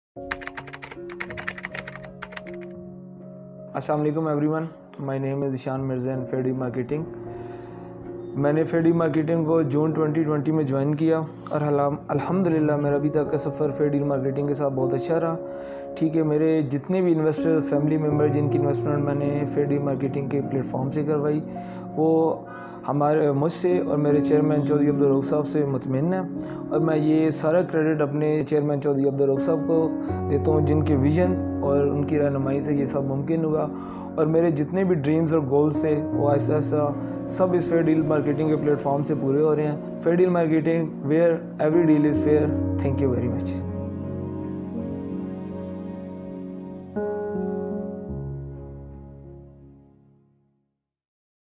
Interviewing Mr. Zeehsan Mirza | Experience & Advice
Mr. Zeeshan Mirza is a real estate professional and a valuable part of Team Fair Deal Marketing.
Watch the video to know what he has to say about Pakistani Real Estate Market and working at Fair Group.